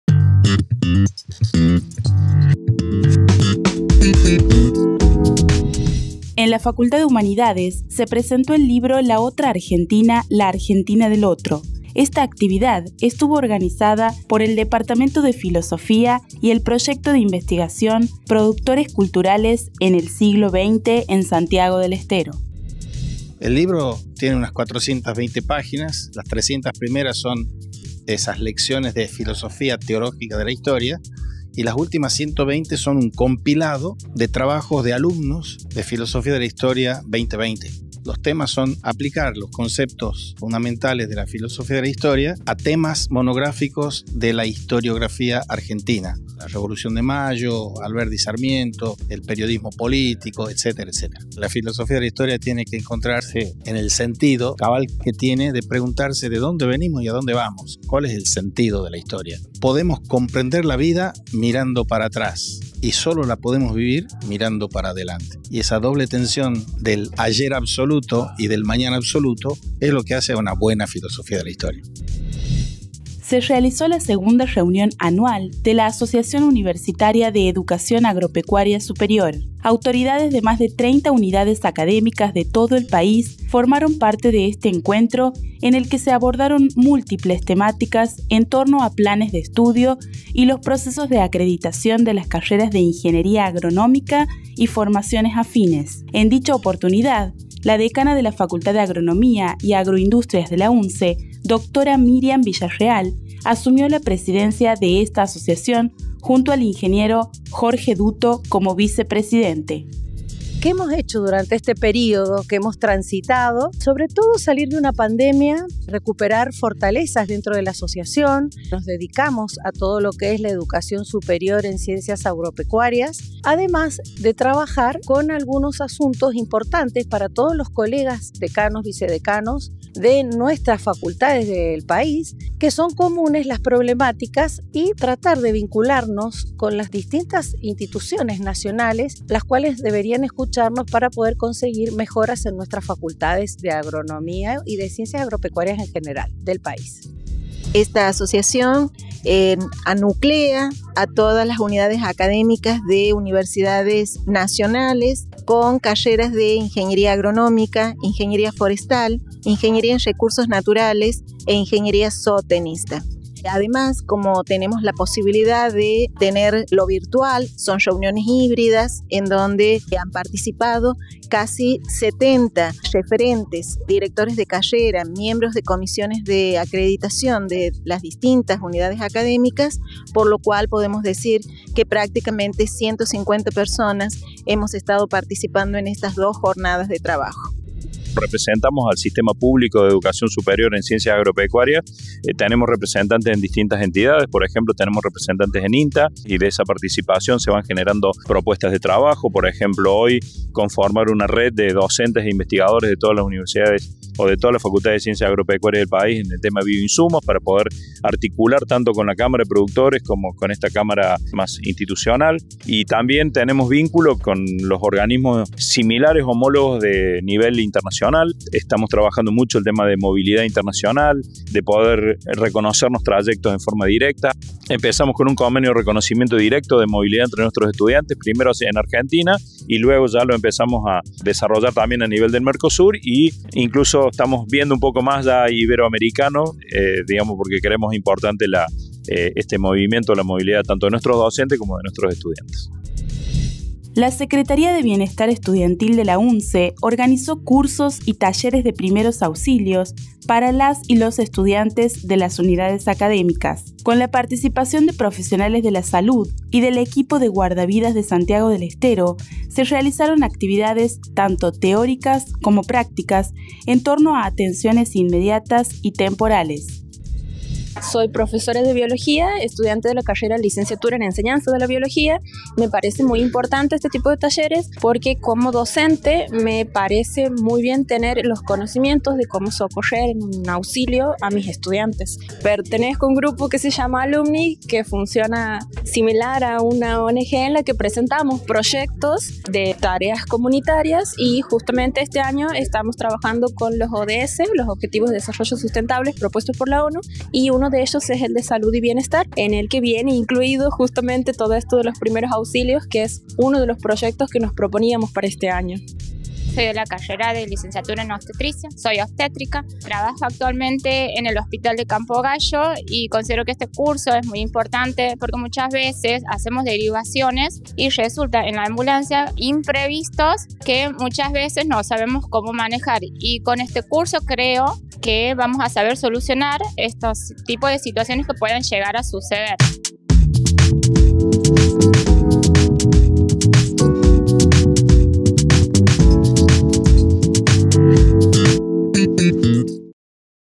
INFO UNSE 05/07/2023
Hoy compartimos información sobre:

🔹 Presentación del libro “La otra Argentina, la Argentina del otro” de Ramón Eduardo Ruiz Pesce.

🔹 2ª Reunión Anual de la Asociación Universitaria de Educación Agropecuaria Superior.

🔹 Cursos y talleres de Primero Auxilios organizados por la Secretaría de Bienestar Estudiantil.